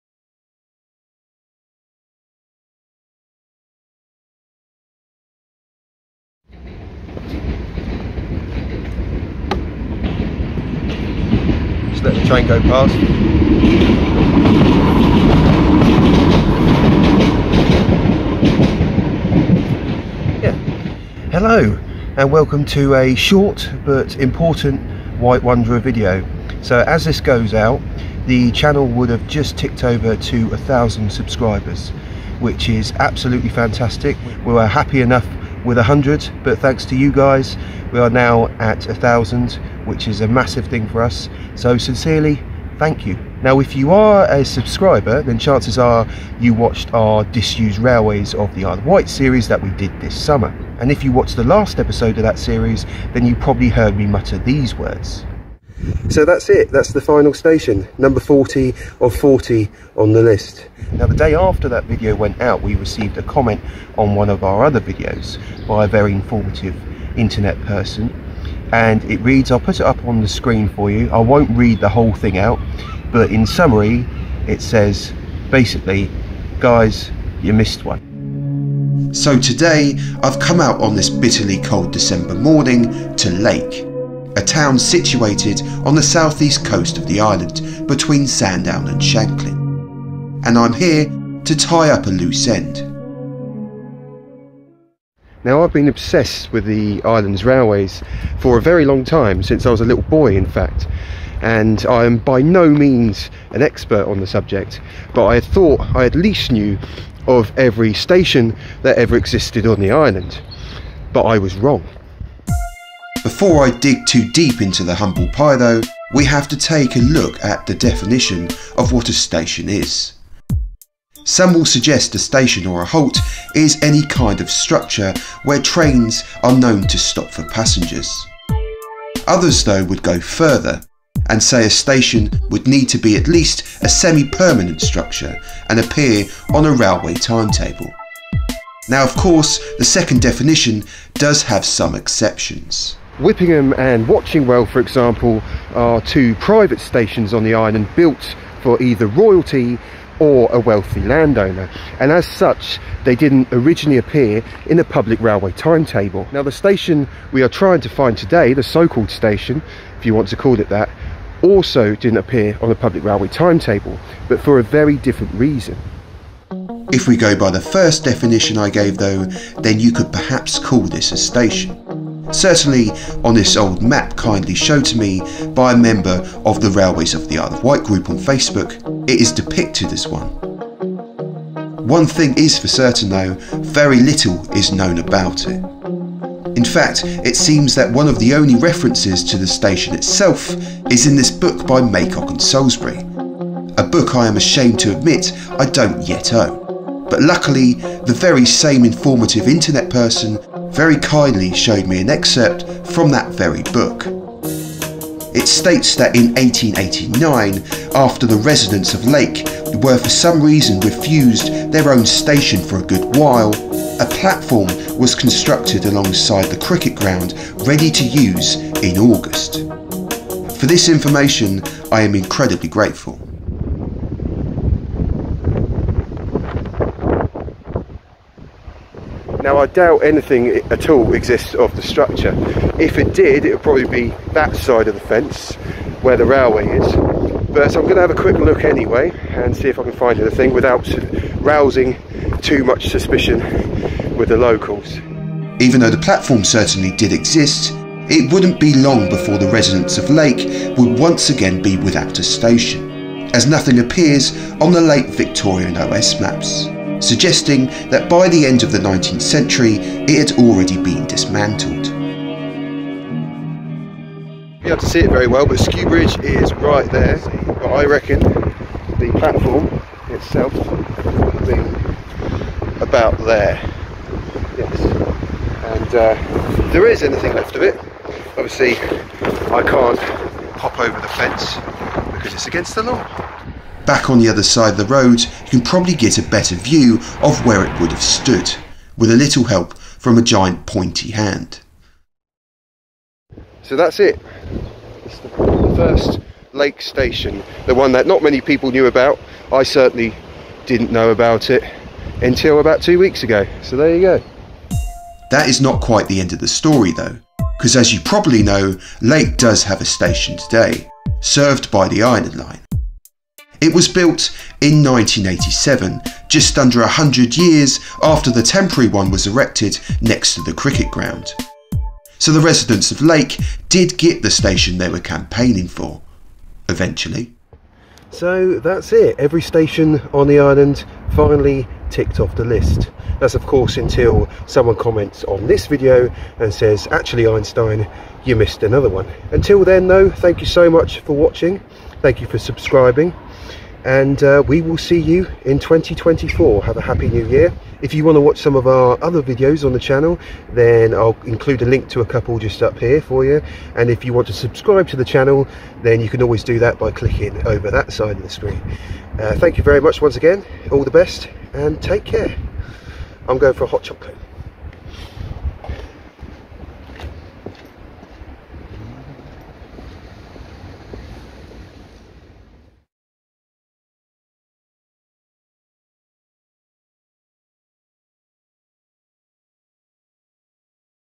The Lost Station in Lake | 1000 Subscriber Special
Searching for the long lost 1st railway station in Lake - 1000 sub special.

A massive thank you from both of us for watching the videos & allowing the channel to grow this year! Here's to a happy 2024.

Special thanks to Francis Stenson on YouTube for 1st alerting me to the subject of a former station on the site.

Special Thanks also to the members of the Railways Of The Isle of Wight group on FB for also contributing information.

Music: all tracks used in this video are composed for the channel by the channel - C: jacksingletonmusic

Image of Medina Wharf Halt - photo by John Law - date unknown.

Maps used: Google Earth 2023.
OS maps published 1899: NLS.co.uk